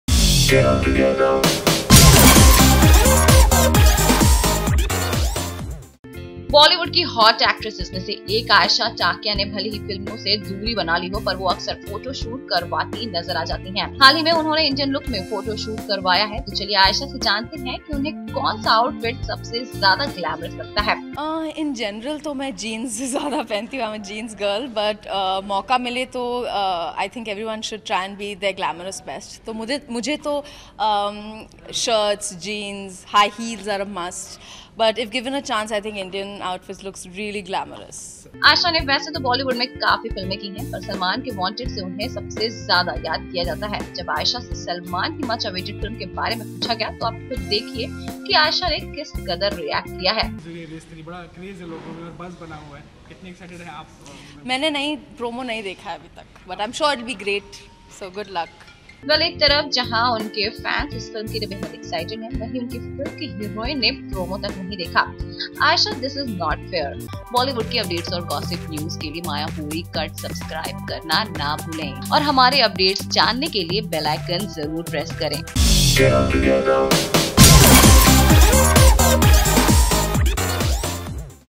"Salman Khan" Ki Heroine Ka Aaya "Race 3" Par Shocking Reaction | Ayesha Takia
बॉलीवुड की हॉट एक्ट्रेसेस में एक आयशा टाकिया ने भले ही फिल्मों से दूरी बना ली हो पर वो अक्सर फोटोशूट कराती नज़र आ जाती हैं। हाल ही में उन्होंने इंडियन लुक में फोटोशूट करवाया है तो चलिए आयशा से जानते हैं कि उन्हें कौन सा आउचफिट सबसे ज्यादा ग्लैमरस लगता है.